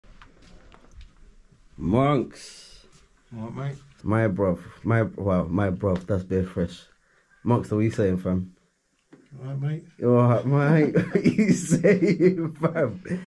New Music Documentary intro 7 - Monkstar
In making and preparing this new music, I’ve caught up with some very key people involved.

Not only will they be speaking about the new music to come but they’ll also be giving you some insights into my music journey, my hustle/grind and how it’s been working together.

Here’s an intro to Monkstar he produced my very first tape.